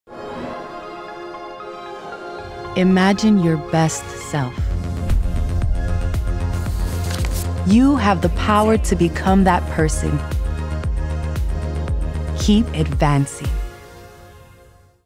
Keep Advancing with Elizabeth Acevedo | Add Instant Radiance
Slam poet. Award-winning author. And she’s just getting started. What will Elizabeth Acevedo accomplish next? ✨ You have the power to do anything – even achieve radiance overnight with AdvancedNightRepair.